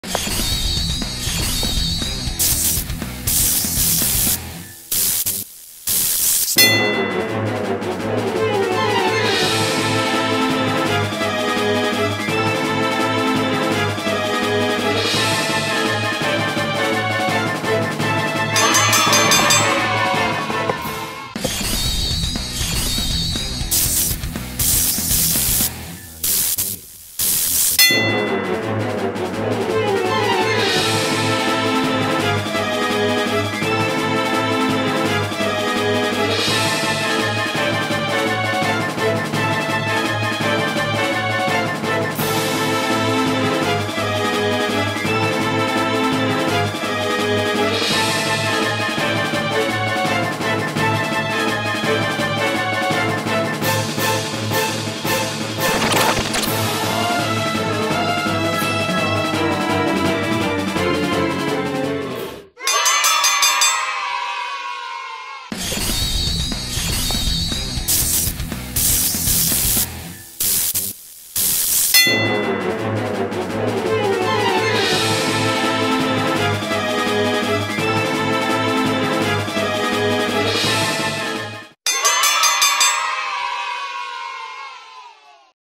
Rematch! Skibidi Toilet VS THE AMAZING DIGITAL CIRCUS in Paper Sumo
Titan Speakerman vs Pomni
Titan TVman vs Jax
Titan Cameraman vs Caine
Who do you think will be the winner?